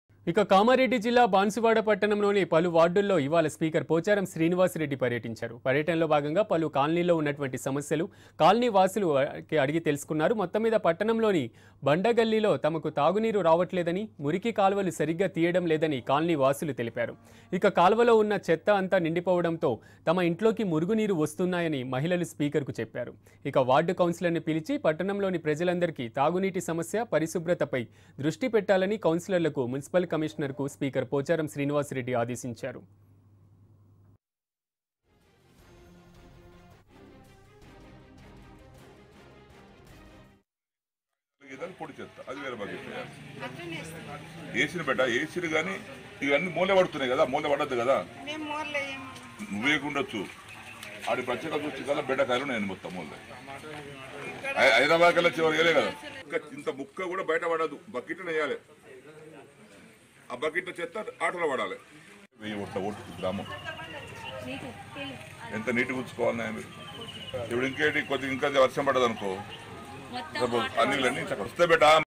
అధికారులకు క్లాస్ పీకిన స్పీకర్ | Speaker Pocharam Srinivas Reddy Inspects Banswada | iNews
Speaker Pocharam Srinivas Reddy Inspects Banswada